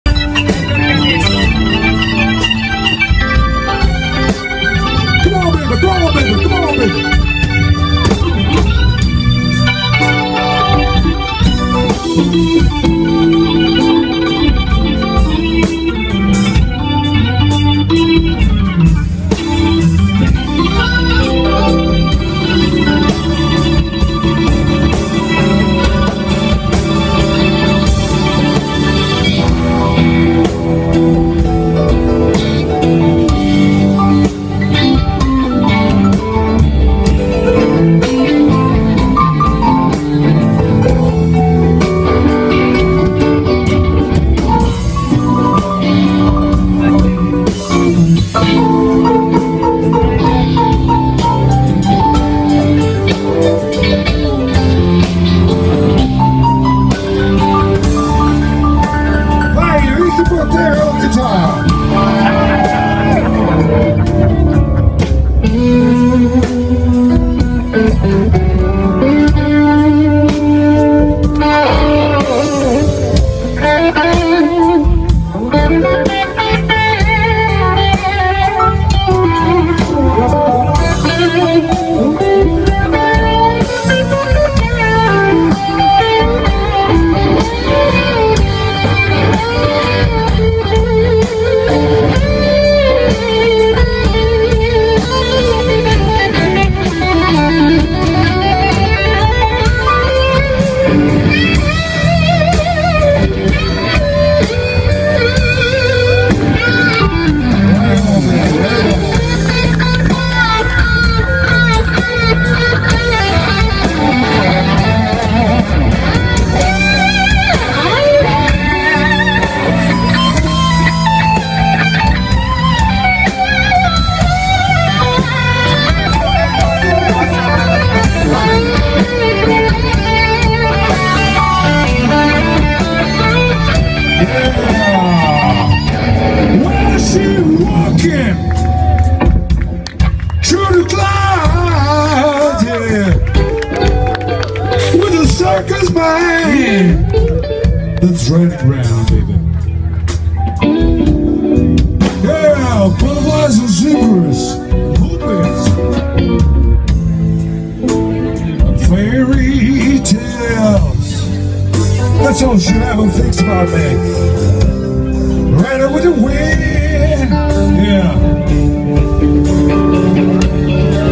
Maurizio Solieri , Alex Cerasani , Mimmo Camporeale , Mister P.Band
Maurizio Solieri ospite dei MISTER P.Band con Mimmo Camporeale , Alex Cerasani, Antonio Lusi , Gabriele Giovenali , Lollo Campani

Beer Fest @ Terni